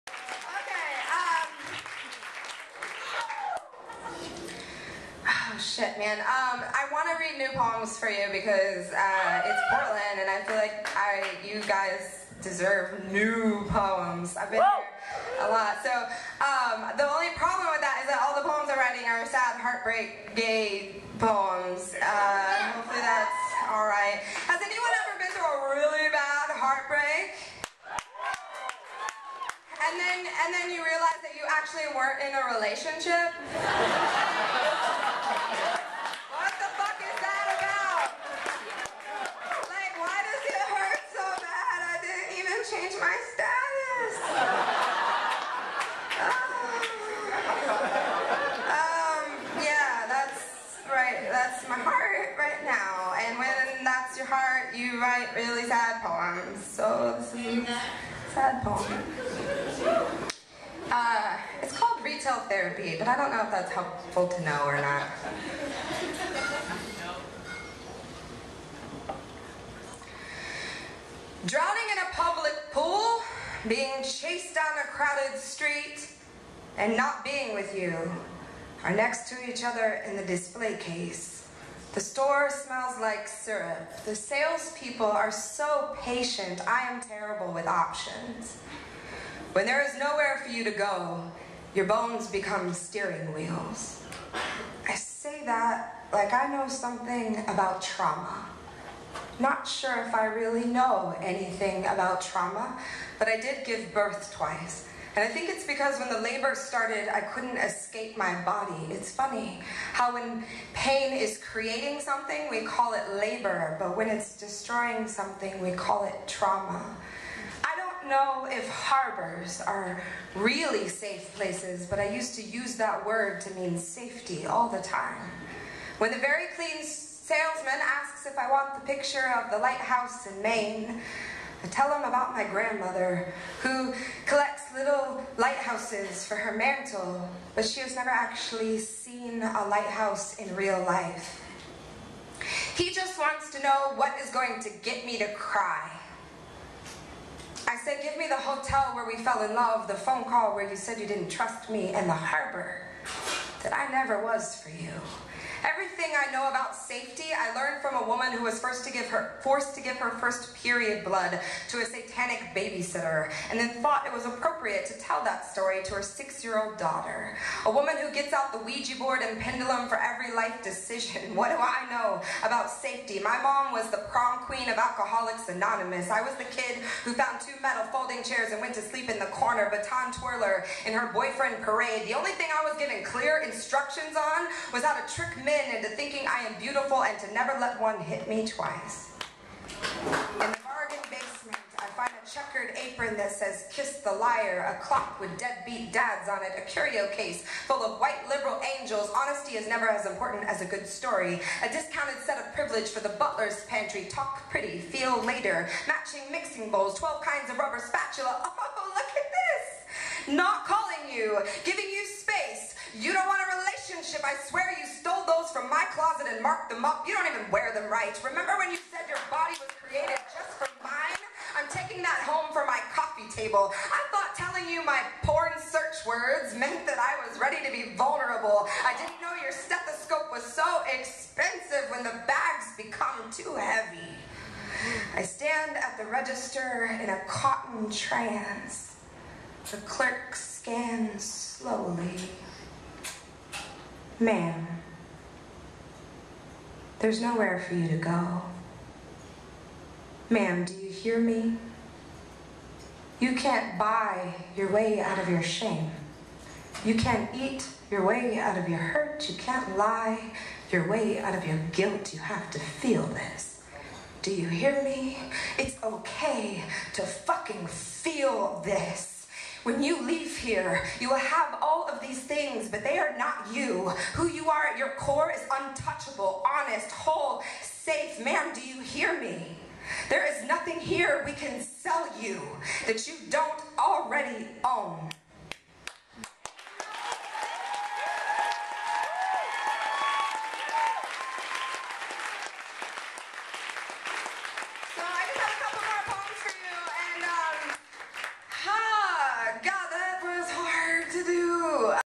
Lauren Zuniga performs a new poem "Retail Therapy" at Portland Poetry Slam
Backspace Cafe. November 18th, 2012. Portland, Oregon.